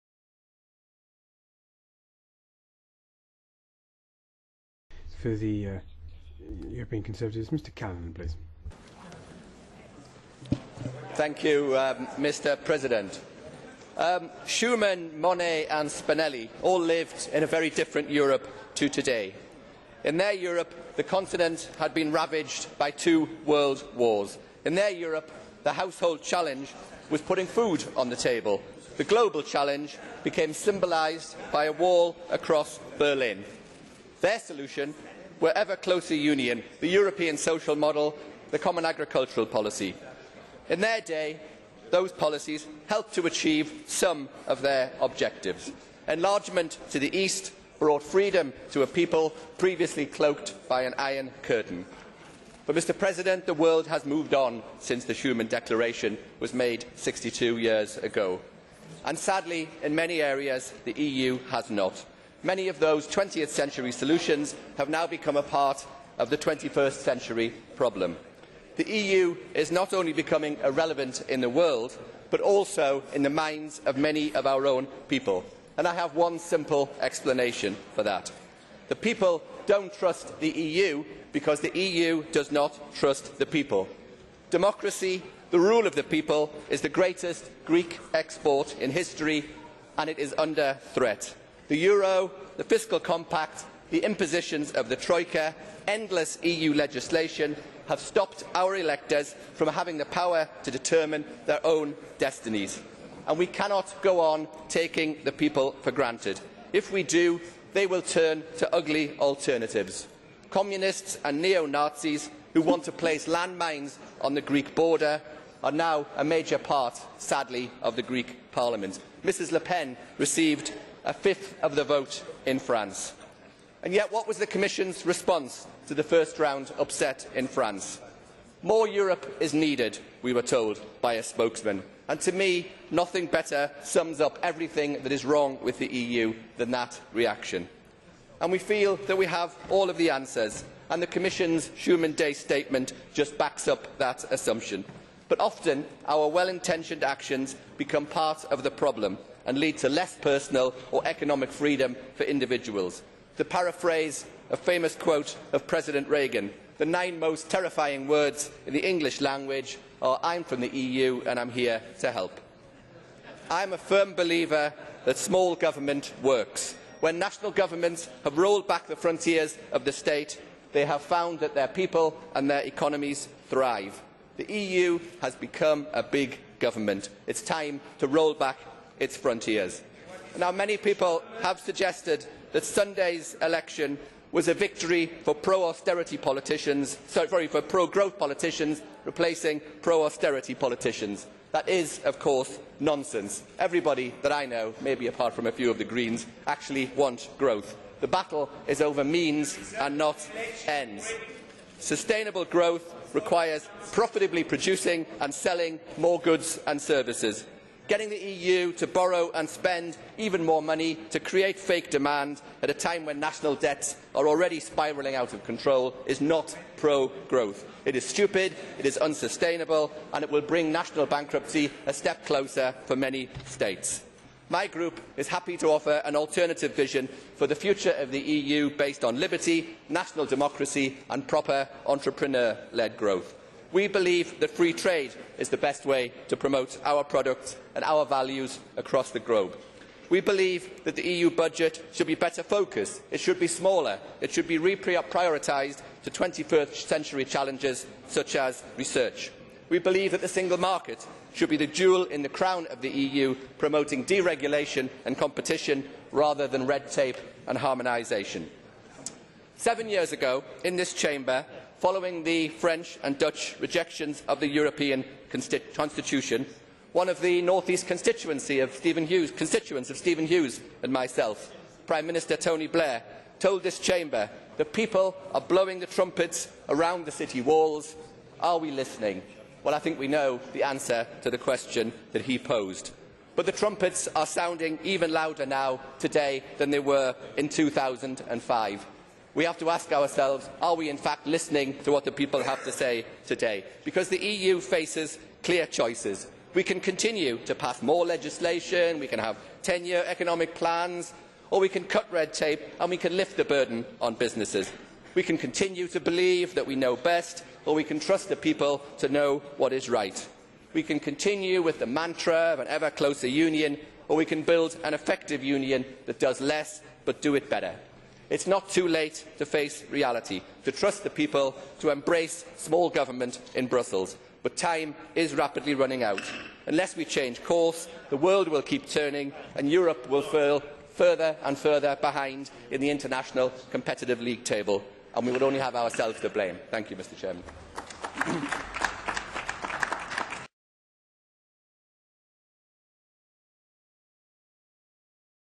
Martin Callanan on the Future of the EU
European Conservatives and Reformists group leader Martin Callanan MEP in a debate with other political group leaders on the Future of the EU. In the debate, held to mark the anniversary of the signing of the Schuman Declaration on 9 May 1950, Callanan warns that the EU has failed to move with the times, offering 20th century solutions to 21st century problems. He warned that the EU's greatest challenge today is to remain relevant in the world, and in the minds of EU voters who the EU has been taking 'for granted', and who are turning to ugly extremes.

Callanan warned of the EU's big government mentality, which believes that only 'more Europe' can solve every challenge, makes it part of the problem. He called for the EU to roll back its frontiers.